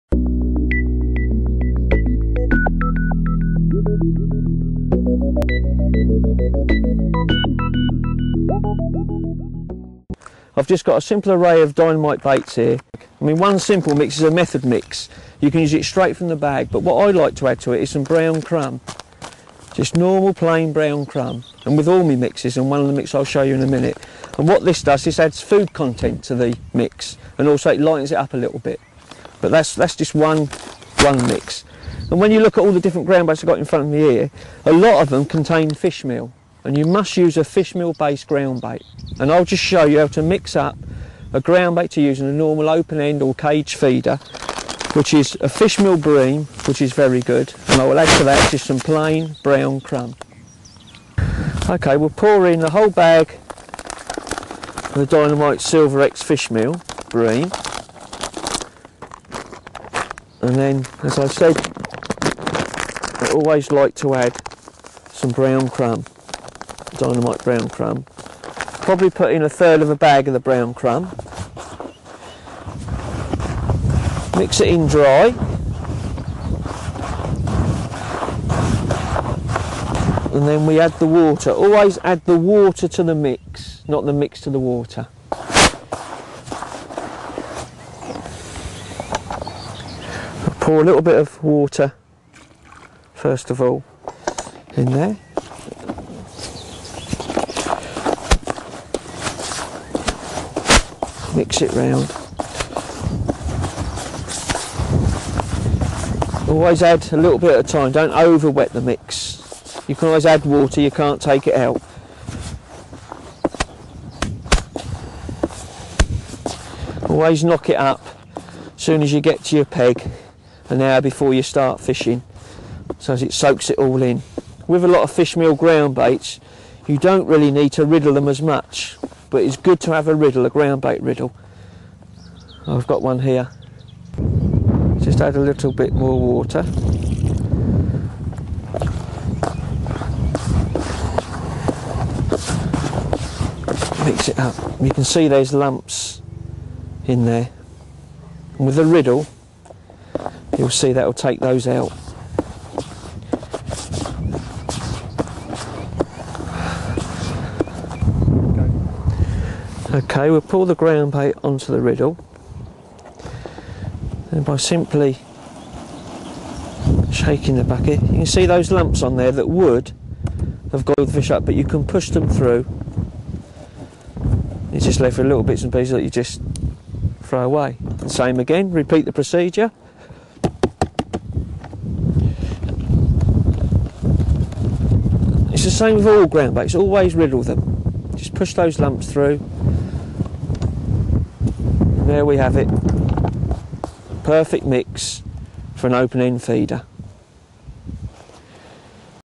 Mix groundbait the right way
Fox Match star Mark Pollard offers a step-by-step guide on the right way to mix groundbait.